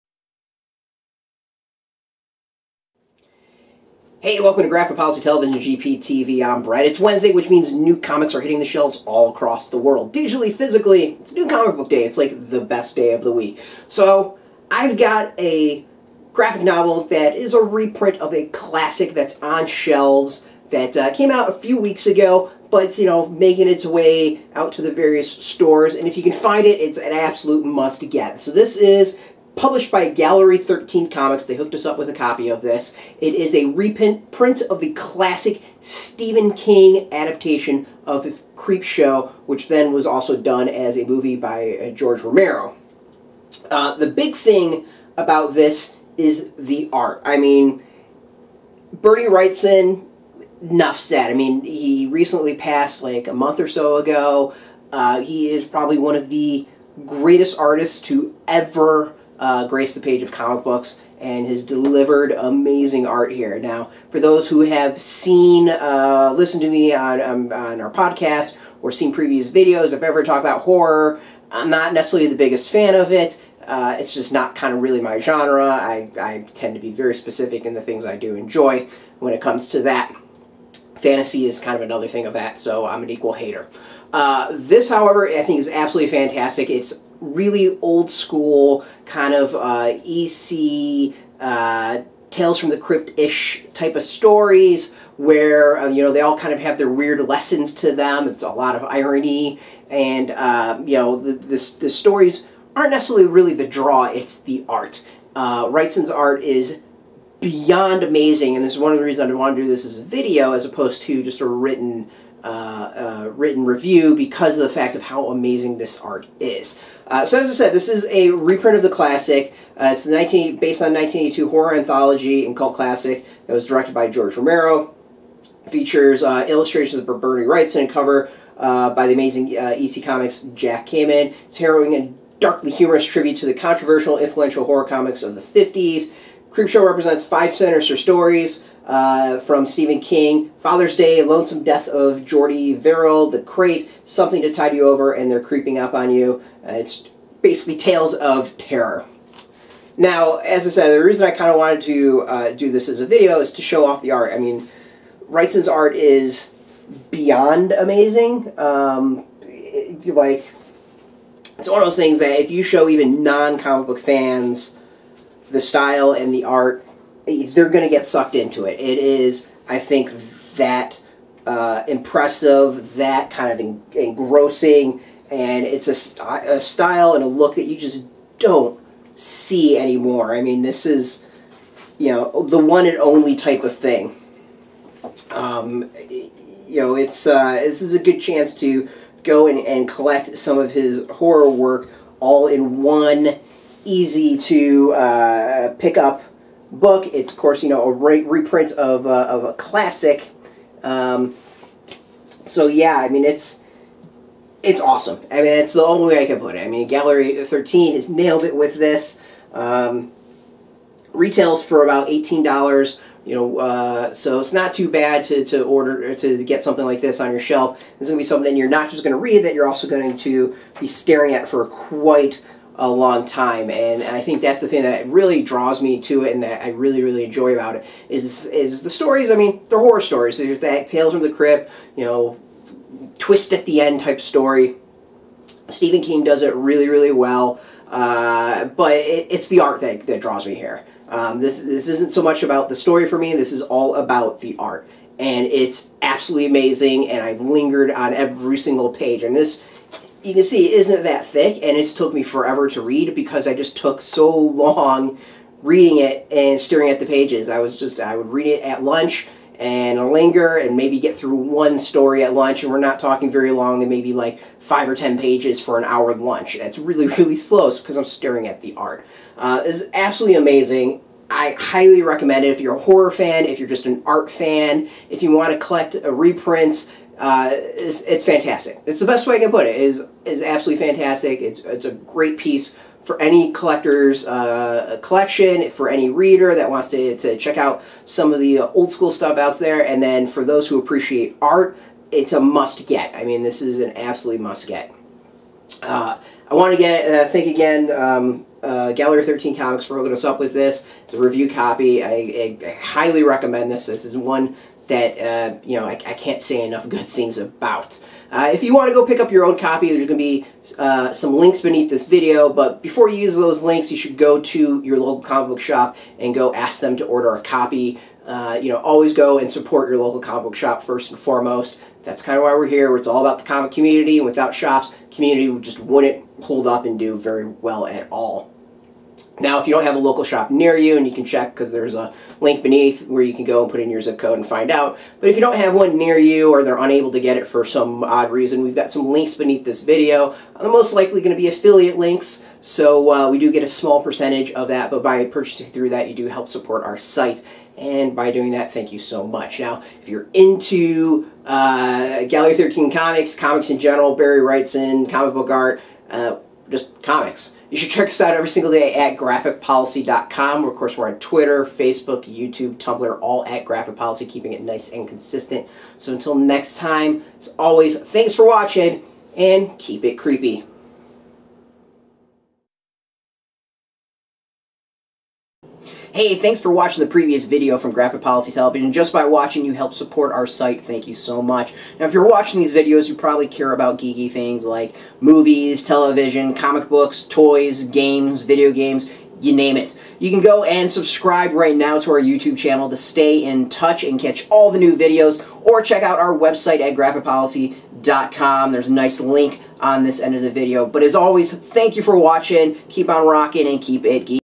Wednesday Graphic Novel Review: Creepshow
The graphic novel adaptation of the classic horror anthology film written by Stephen King, with art by Bernie Wrightson!

Now back in print: the graphic novel adaptation of Stephen King’s Creepshow, based on the 1982 horror anthology and cult classic film directed by George Romero (Night of the Living Dead, Dawn of the Dead)—and featuring stunning illustrations by the legendary Bernie Wrightson and cover art by the acclaimed Jack Kamen! A harrowing and darkly humorous tribute to the controversial and influential horror comics of the 1950s, Creepshow presents five sinister stories.

This is a graphic novel that's a must get and we tell you why!

Gallery 13 Comics provided Graphic Policy with a FREE copy for review